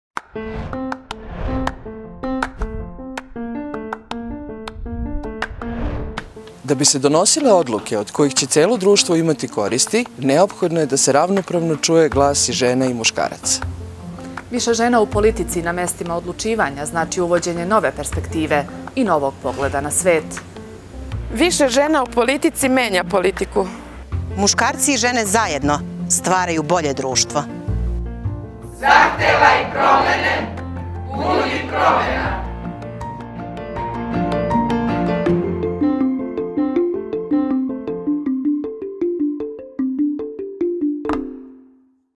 2EUzaŽENEZlatiborskogokruga
Често чујете да жена нема на местима одлучивања зато што не желе да се баве политиком. То није тачно!
Чињеница је да постоје бројне препреке за веће учешће жена у политици.
Битно је да се у Парламенту, локалним скупштинама, саветима месних заједница чује глас жена јер оне имају различите потребе од мушкараца.
 И ко ће боље заступати потребе жена од њих самих?